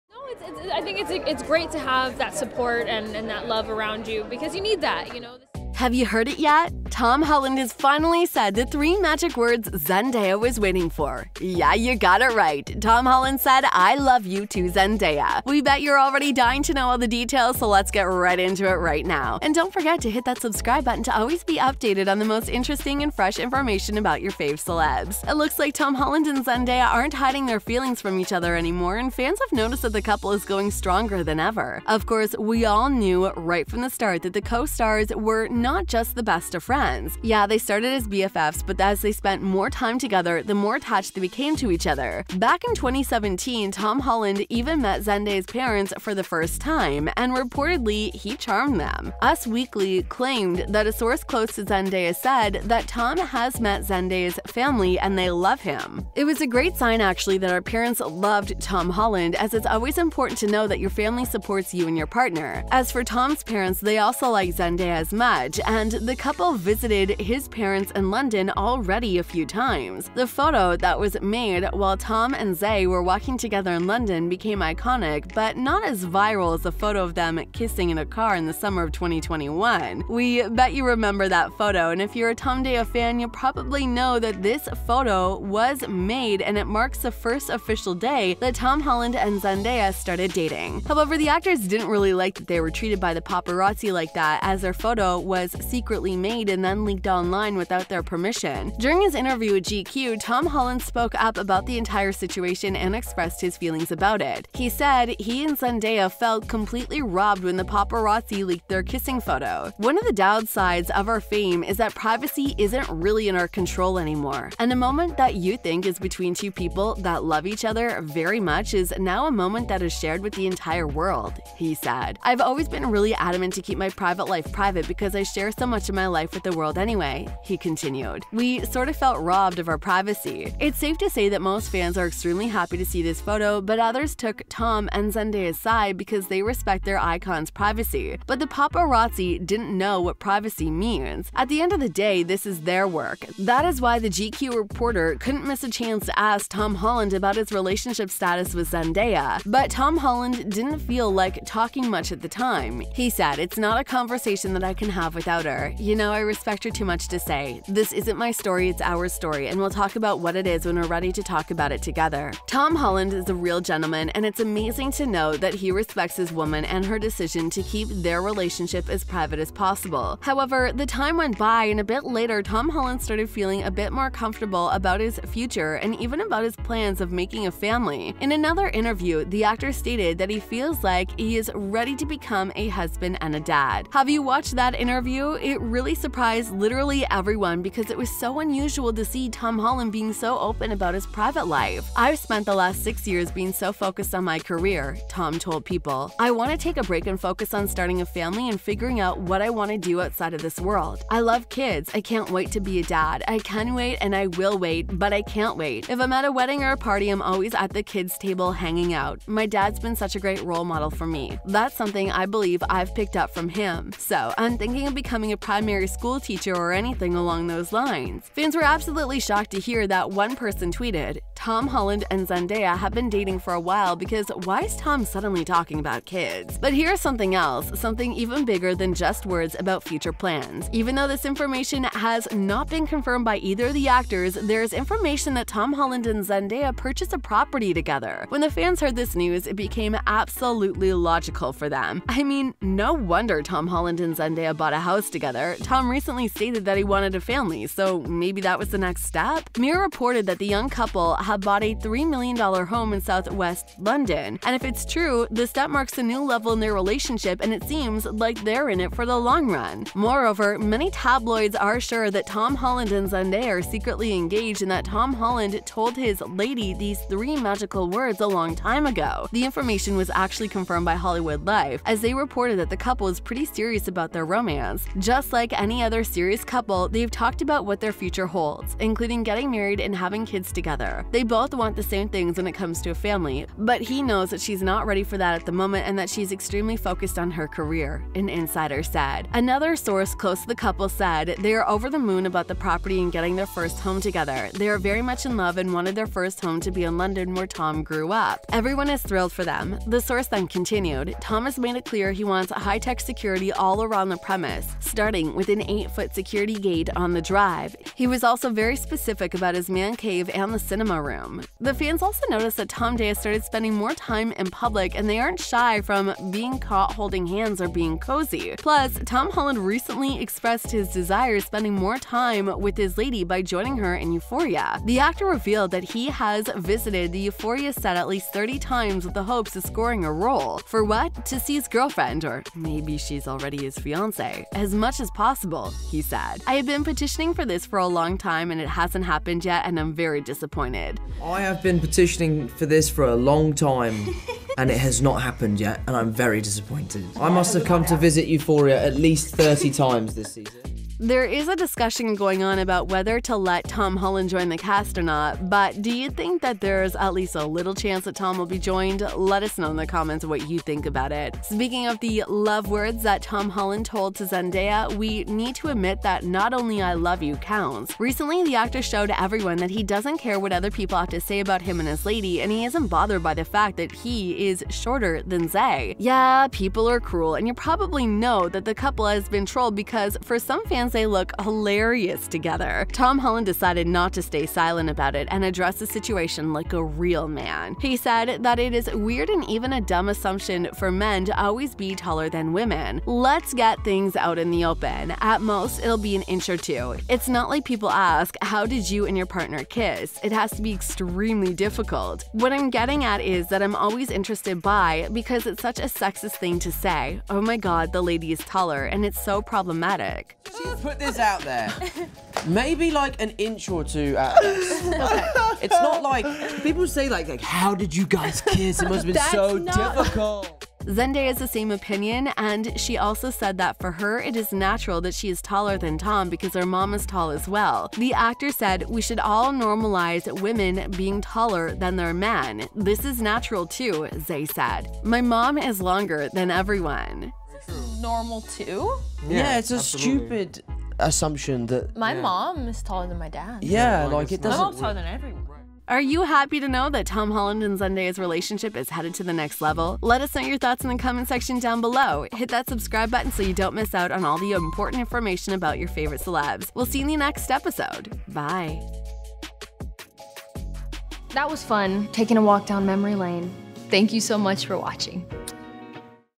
"I Love You" Tom Holland Finally Said The Three Magic Words To Zendaya!
Tom Holland Finally Said The Three Magic Words To Zendaya!

Have you heard that? Tom Holland has finally said the three magic words Zendaya was waiting for! Yes, you got it right, Tom Holland said ‘I love you’ to Zendaya. We bet you are already dying to know all the details so let’s get into it right now!